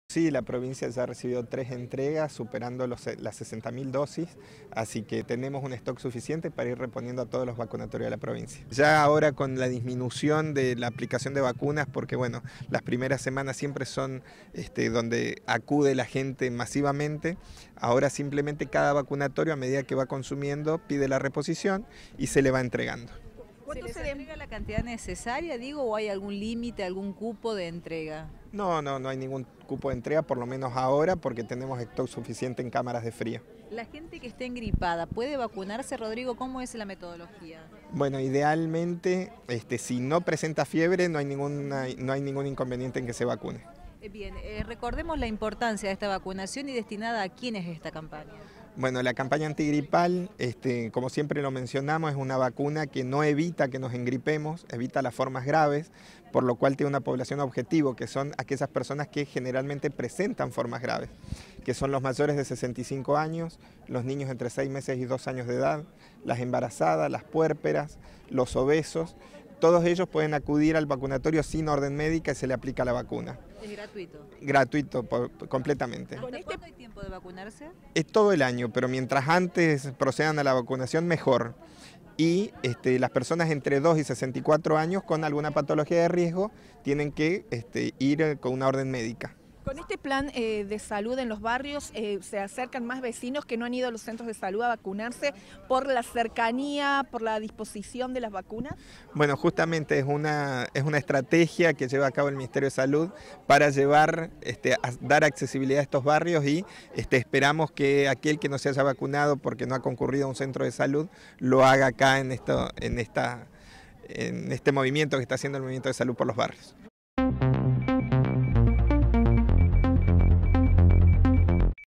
23-05-2019///CONTINÚA LA CAMPAÑA DE VACUNACIÓN ANTIGRIPAL GRATUITA
RODRIGO VERDUGO - EPIDEMIOLOGÍA.-
SE ENCUENTRAN DISPONIBLES EN TODOS LOS VACUNATARIOS Y CENTROS DE SALUD LA VACUNA CONTRA LA GRIPE.-